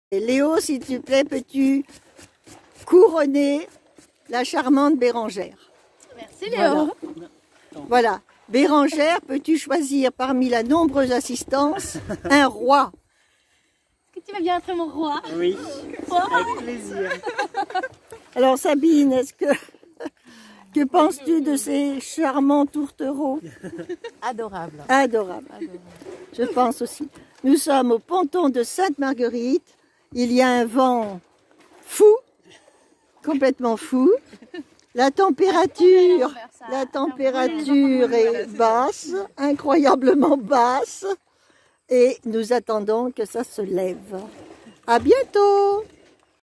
2021 janvier La Galette des Rois à Sainte Marguerite
En attendant le vent PASSION III a fait escale à Sainte Marguerite pour un excellent déjeuner qui s' est terminé par une galette des rois à la parisienne